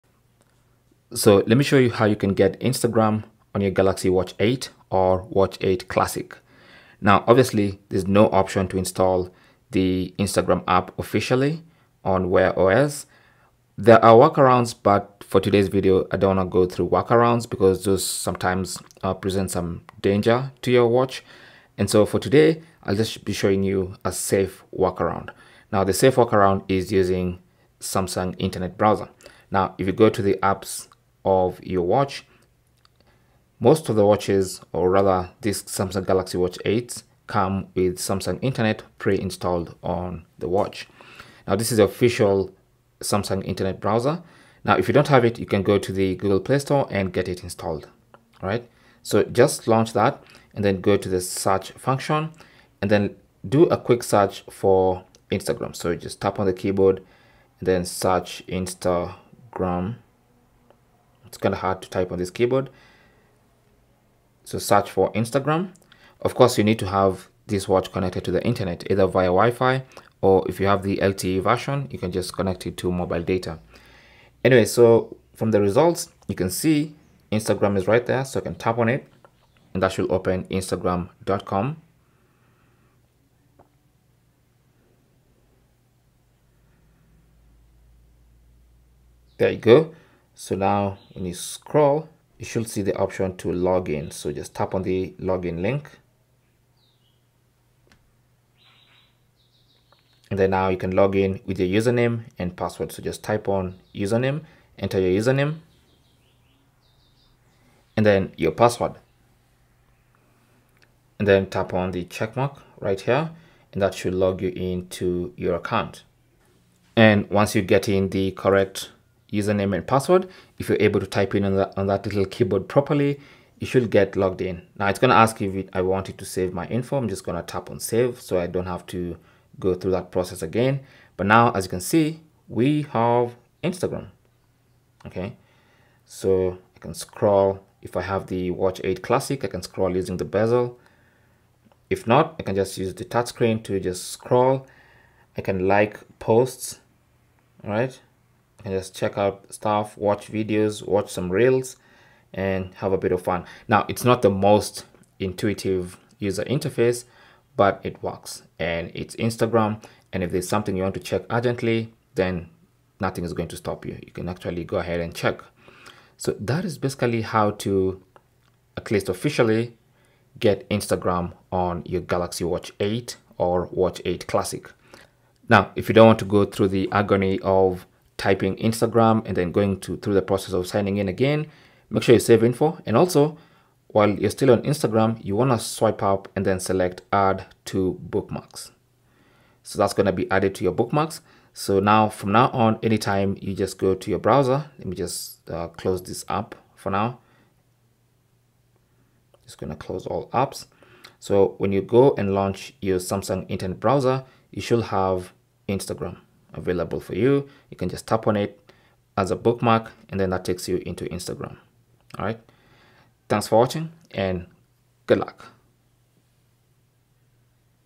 How To Use Instagram On Samsung Galaxy Watch 8 / 8 Classic
In this video, I’ll show you how to use Instagram on the Samsung Galaxy Watch 8 and Galaxy Watch 8 Classic.

You’ll learn to check DMs, likes, stories, and more without pulling out your phone.

Samsung Galaxy Watch 8 Classic

How To Open Instagram On Samsung Galaxy Watch 8 / 8 Classic:

Your Galaxy Watch 8 runs on Wear OS, which means you can install a browser app (like Samsung Internet for Watch). Through this, you can open Instagram directly.

Step 1: If the Samsung Internet browser is not installed on your Galaxy Watch 8, launch the Google Play Store and search for Samsung Internet Browser, then install it.

Ensure your watch is connected to Wi-Fi or mobile data if you're using the LTE version of the watch.

Step 2: Open the browser from the watch's app settings, tap on the search function and type Instagram using the watch's keyboard. Select the instagra.com link.

Step 3: Tap on the "Log In" link to your account with your username and password. Then tap on "Log In".

Step 4: You will be prompted to save your log in info. Tap on "Save Info" if you will be accessing Instagram often on your Galaxy Watch 8.

Now you can scroll through posts, like them, comment, view stories, or exploring profiles without your phone, right from your wrist.

Step 5: Swipe up from the bottom of the watch screen and select "Add to Bookmarks". This will save the bookmark to the Samsung internet screen where you can access it quickly.

Cell Phone Tripod Adapter